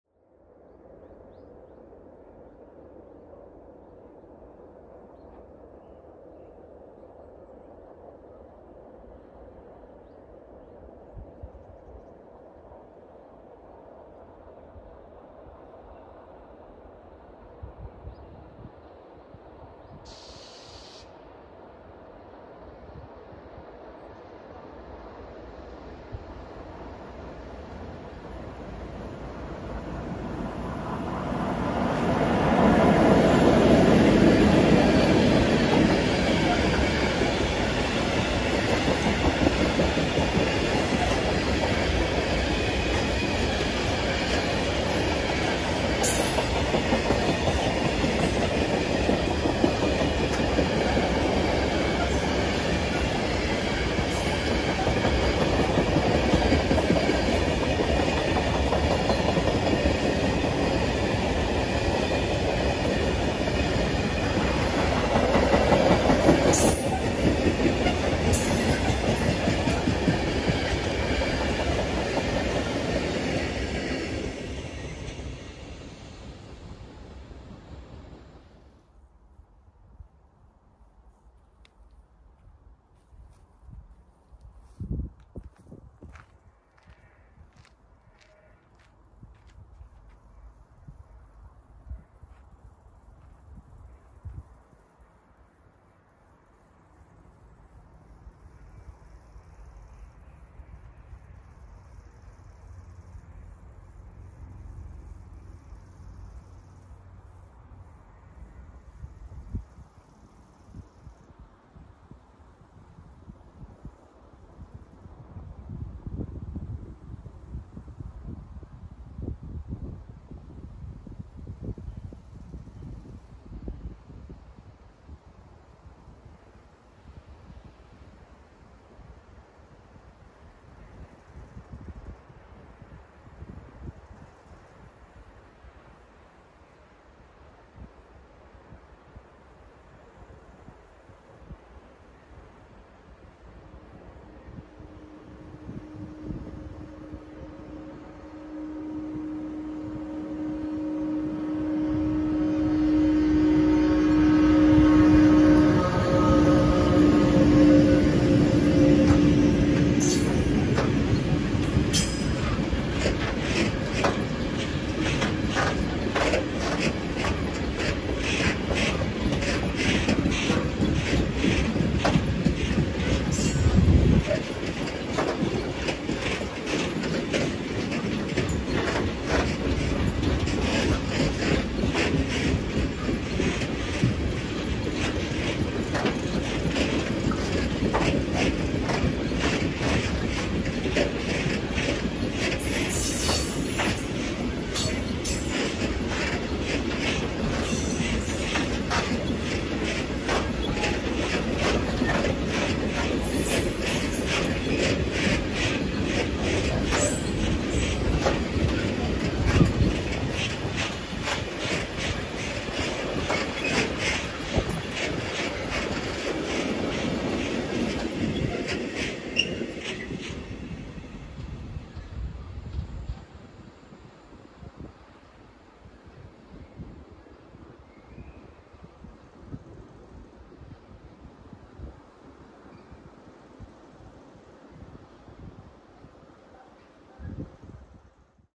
[ČDC] Prawie jak Škoda 131 "dvojička" czyli duet(y) "Pershing" -ów 163 do Połańca [4K]
Ku zaskoczeniu wielu osób (w tym mnie), zamiast tradycyjnego duetu Škód 182, Dragonów I/II, Traxxa 2/DC3 targającego ciężkiego ładownego "Połańca" lub mixów tych lokomotyw, spółka pozwoliła sobie w mojej i wielu opinii na dość ciekawy i odważny krok w postaci puszczenia ładownego ciężkiego (około 3300 ton) "Połańca" z duetem lokomotyw Škoda 163.

Miła odmiana a cieszy.

SPIS:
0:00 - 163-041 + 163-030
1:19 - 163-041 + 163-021

Tradycyjnie, Pozdrowienia do obydwu drużyn trakcyjnych oraz Podziękowania dla informatorów.